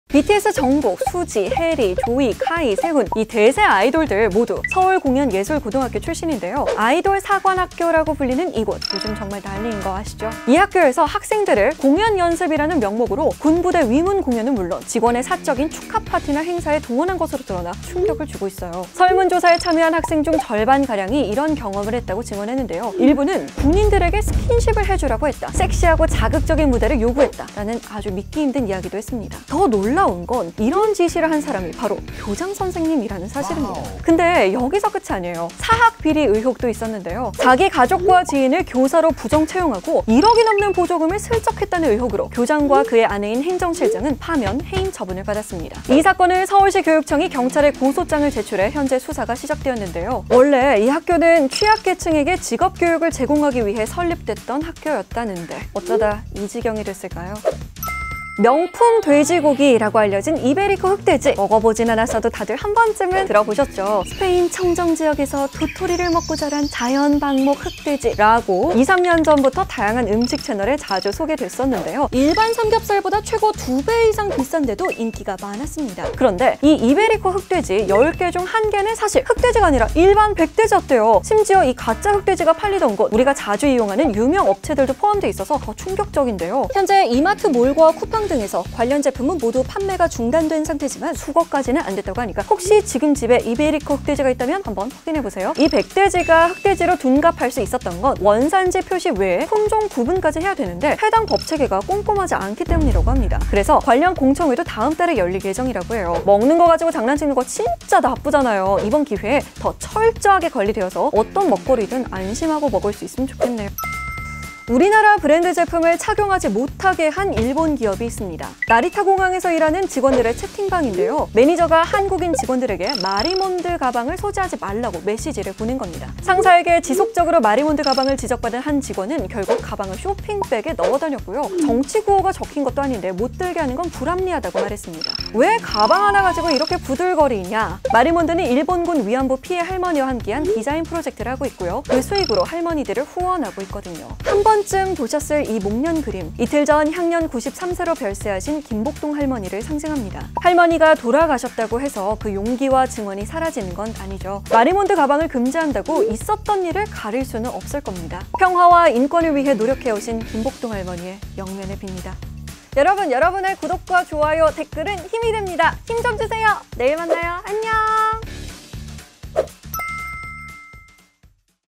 서울공연예술고의 사학비리, 학생동원 의혹 / 14F
술자리에 '학생'을 동원한 고등학교가 있다